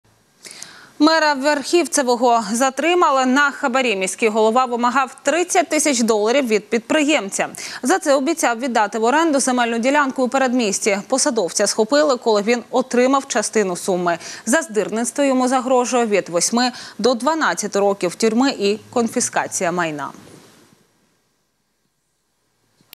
Мэр Верховцево попался на взятке в 30 тысяч долларов
Мэра Верховцево задержали на взятке. Городской голова требовал 30 тысяч долларов от предпринимателя. Подробности